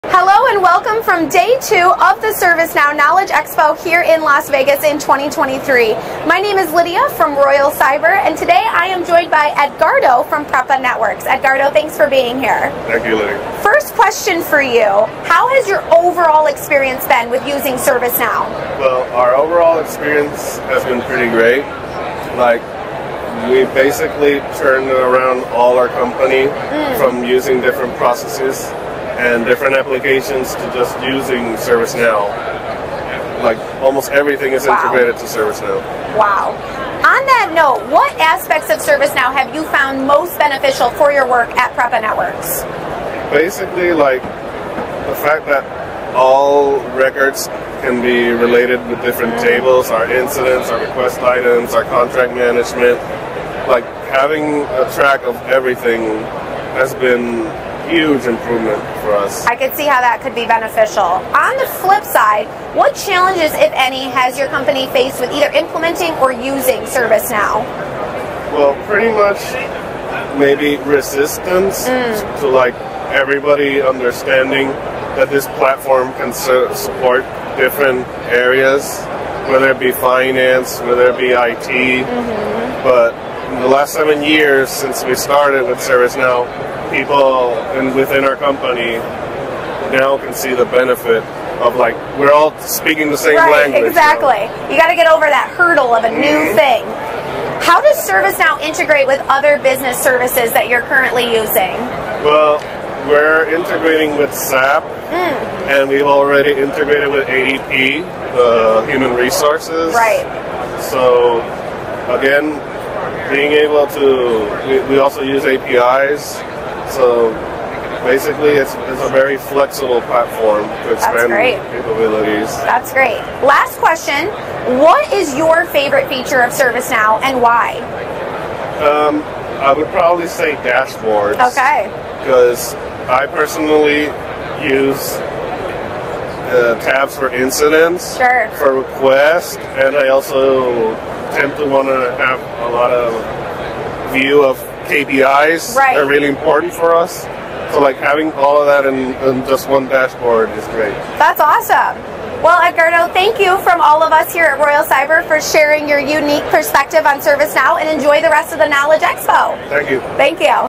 ServiceNow Knowledge Conference 2023
Meet Edgardo from Prepa for our ServiceNow Knowledge 2023 interview series. In this video series, watch Royal Cyber representative Lydia in conversation with experts across industries covering a range of topics:

✅ Their firsthand experience working with ServiceNow,
✅ The benefits of the ServiceNow Platform,
✅ ServiceNow standout features,
✅ Challenges/potential areas of improvement.
Watch and gain insights from Edgardo's ServiceNow journey.